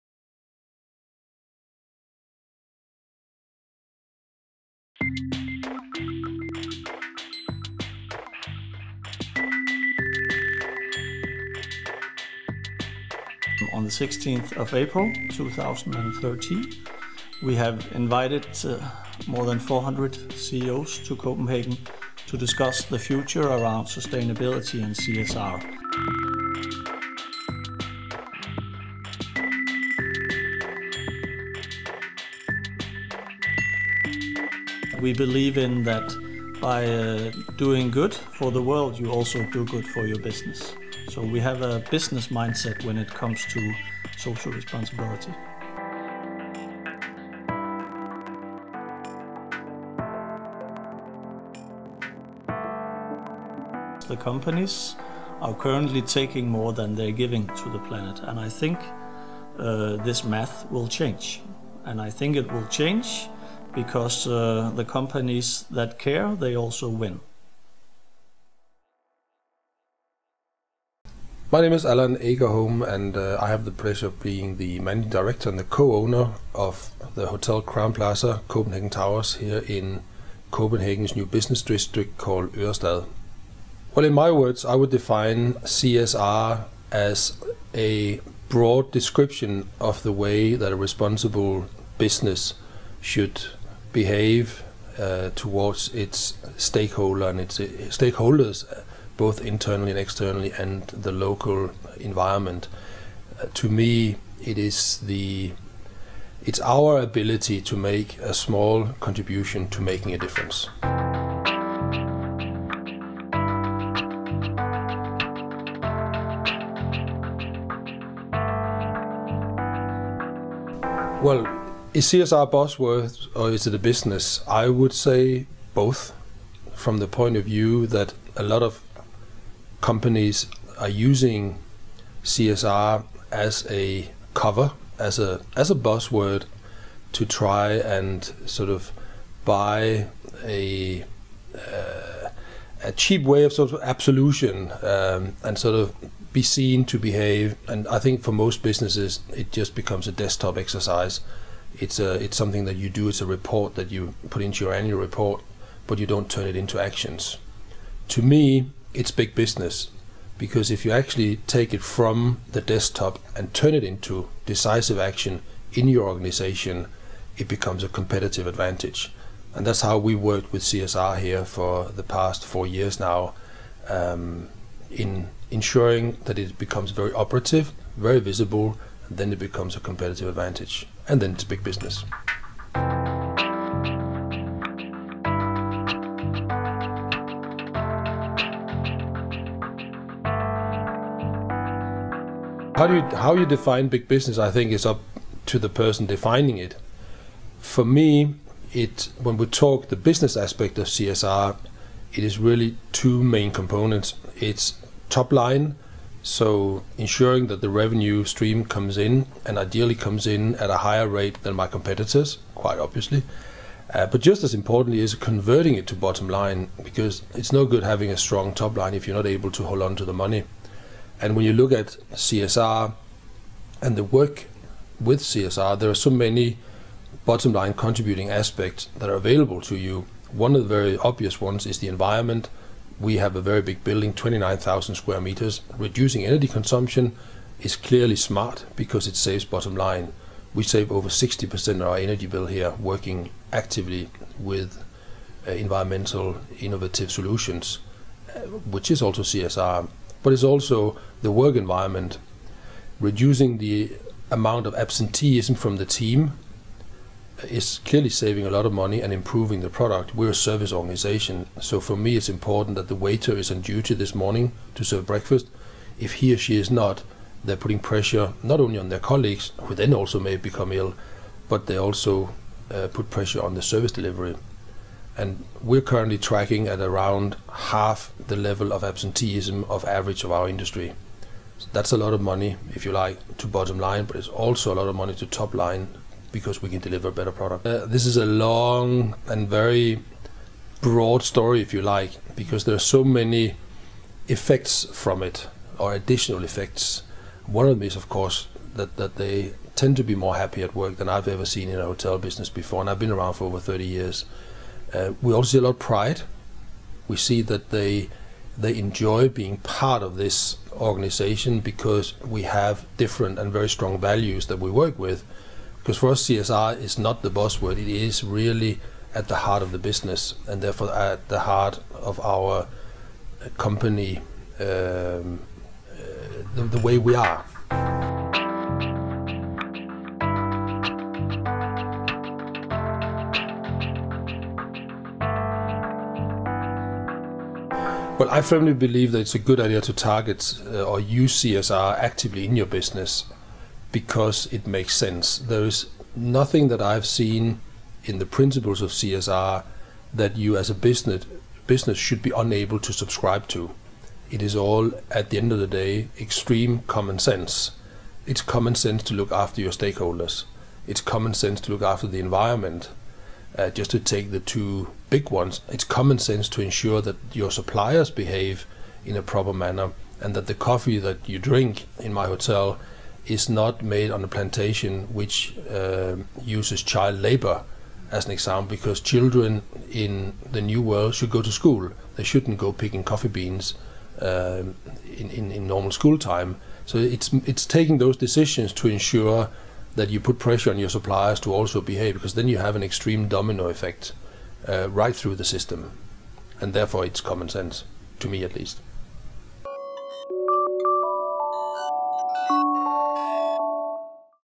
LedelsesTV - Allan L. Agerholm, Hotel Crowne Plaza Copenhagen Towers (1:2) - Who Cares Wins
This series of interviews made for Who Cares Wins, are centered around the topic Corporate Social Responsibility - how danish companies work hands on with bringing CSR strategies to the front of their business. In this episode we have visited Allan L. Agerholm, the managing director of the worlds most eco-friendly hotel, Hotel Crowne Plaza Copenhagen Towers.